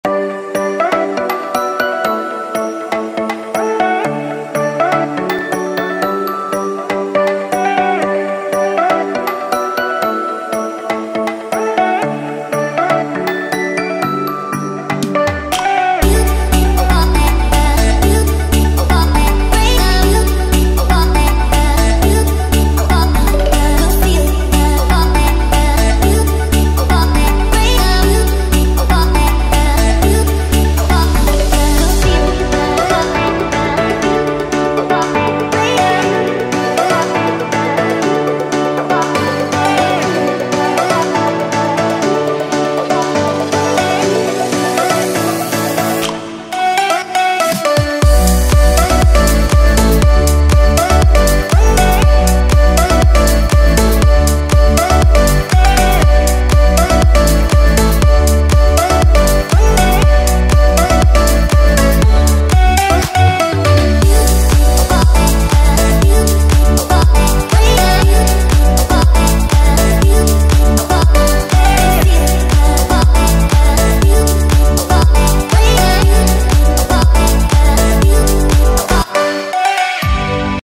Mailbox installation
Use slab paver as the foundation and make sure level it. And drill the hole on the paver . In the end, use anchor lock the mailbox on the paver .